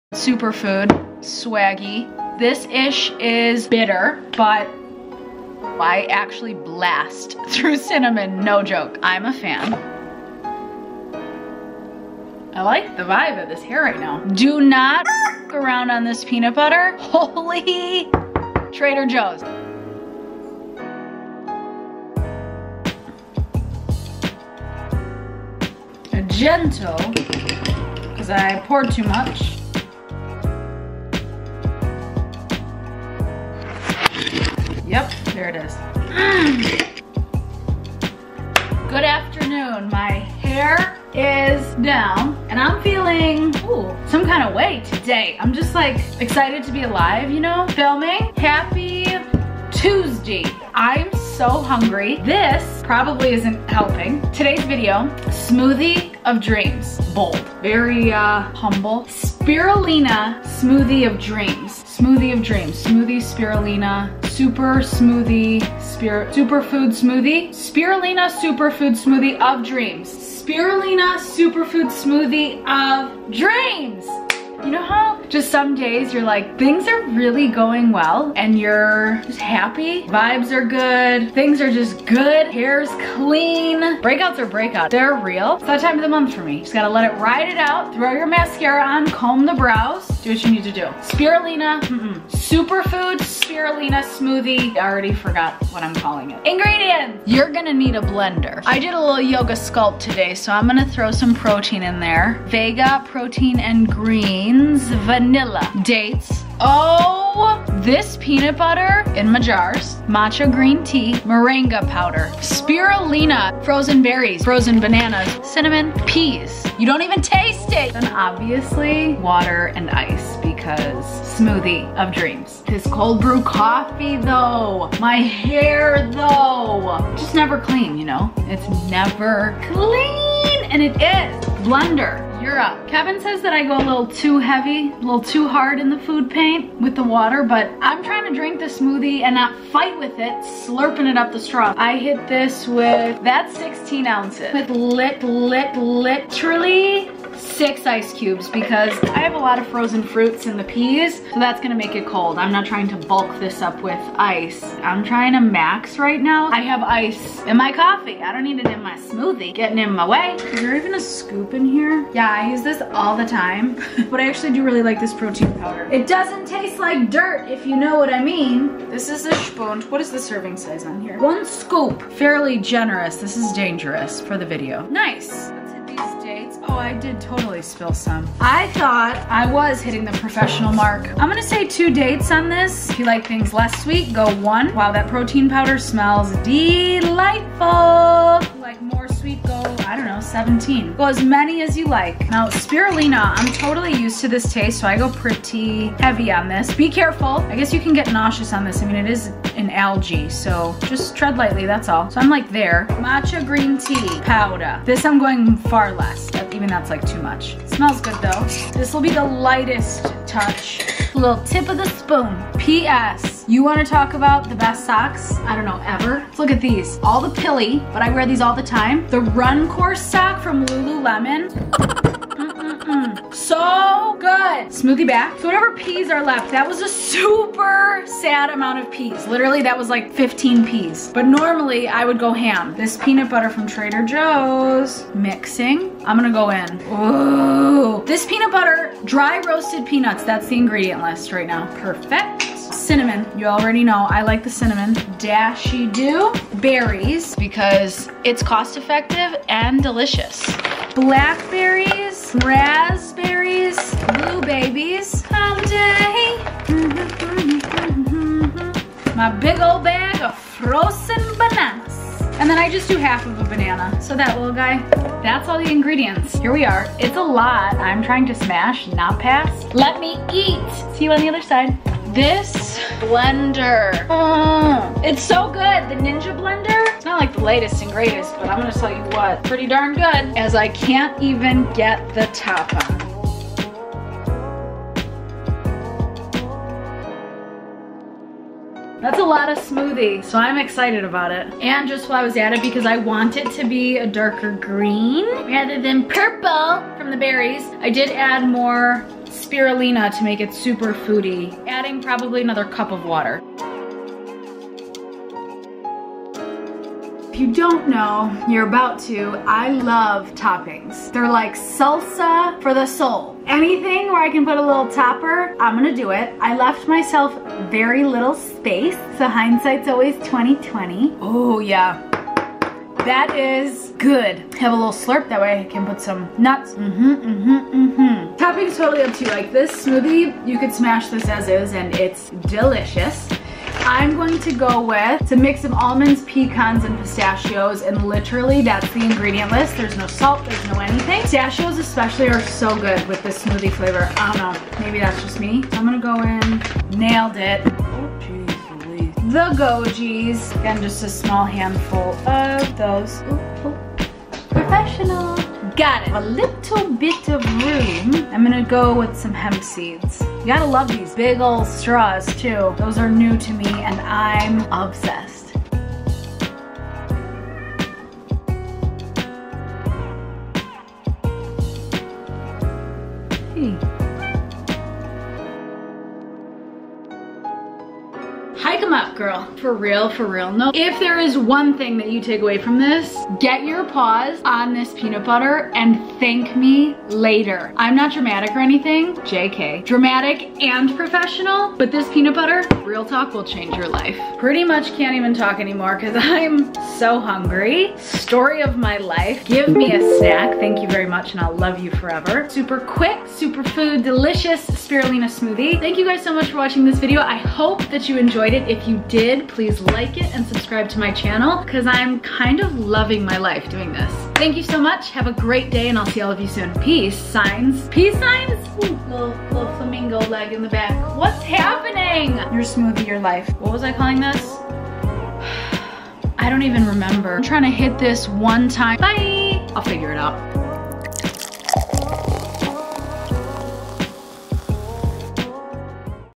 COOK WITH ME! | Spirulina Superfood Smoothie of Dreams
When the superfood shoe fits, you make a spirulina smoothie with me and call it a GREAT DAY. Let's get professional, shall we?

Let's be friends on the gram - @tobecaitlin

Be kind to yourself, be kind to others.